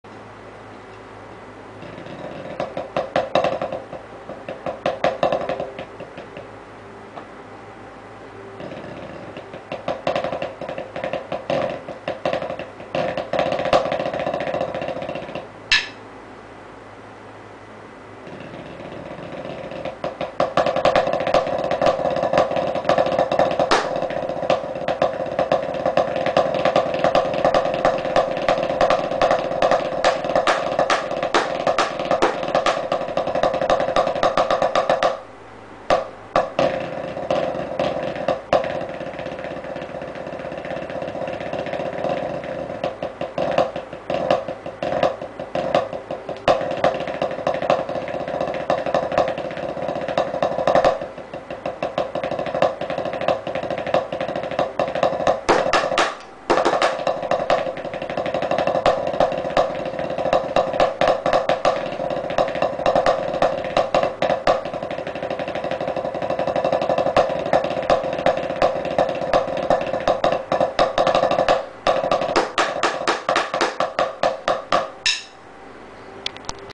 Cadets 2002 Drum Break
This begins right after the opener, during the exchange between brass/drumline sections, and follows through the rest of